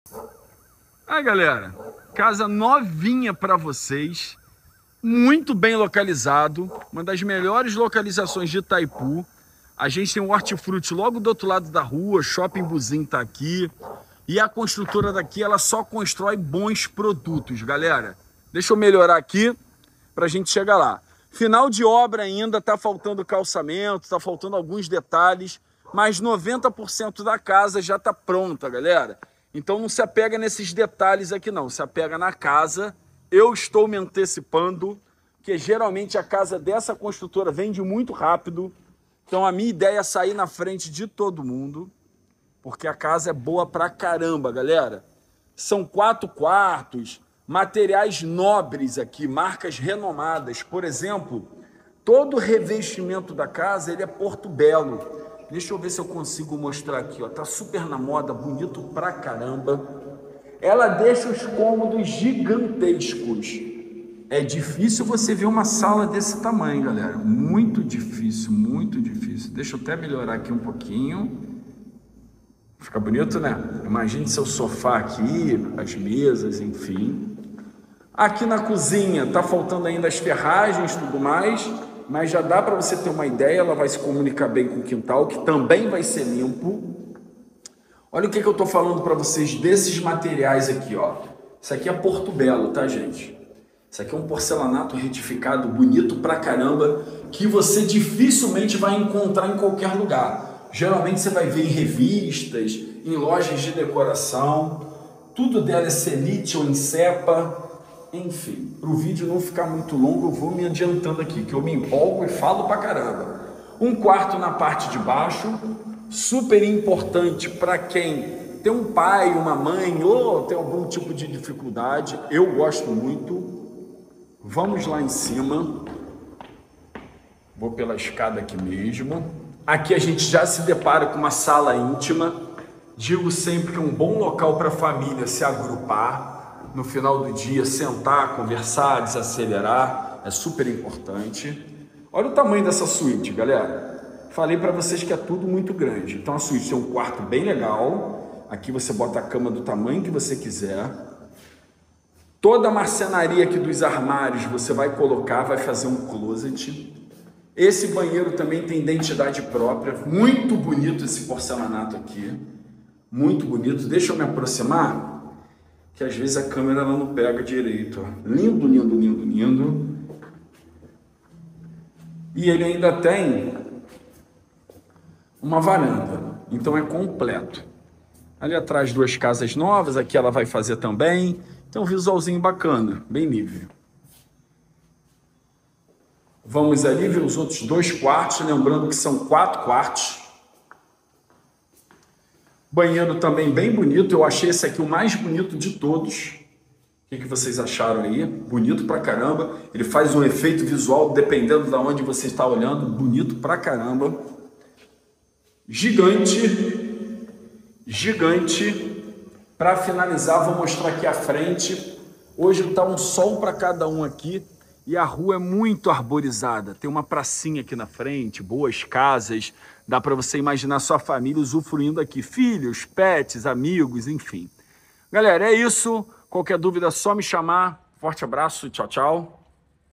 Rua fechada ao lado do Restaurante Buzin🔝
Características principais do imóvel
• Valor do imóvel: R$ 900.000,00
• Valor do IPTU: R$ 248,00 mensal
• Metragem: 175m² de área construída e 180m² de terreno
• Código do imóvel: CA6449

Situada em uma rua fechada exclusivamente residencial em Itaipu, Niterói, esta casa, construída por uma renomada construtora da região oceânica, é um verdadeiro tesouro. Com 180 m² de quintal e 175 m² de construção, a residência apresenta um design contemporâneo, evidenciado pelo revestimento em porcelanato que percorre seus ambientes.

A proximidade com o famoso restaurante Buzin e comércios de destaque confere praticidade e prestígio à localização. Com quatro quartos, incluindo uma suíte, distribuídos entre os dois pavimentos, a casa oferece espaços amplos e funcionais, como sala, cozinha e banheiro. O segundo pavimento conta ainda com uma sala íntima, proporcionando um ambiente aconchegante para momentos de convívio.

Para quem busca uma residência completa, em uma região com escolas conceituadas, essa casa é uma oportunidade única. Com o valor de 900 mil, a imobiliária Luis Imóveis é a responsável pela venda, representando a chance de adquirir um lar especial em meio a essa atmosfera exclusiva e familiar.

Rua Jefferson Rocha, 165 - Rotatória da Av. Central - Itaipu - Niterói/RJ
Estrada Francisco da Cruz Nunes, 580 - Barravento - Piratininga - Niterói/RJ

Facebook.com/luisimoveis - Instagram: @luis_imoveis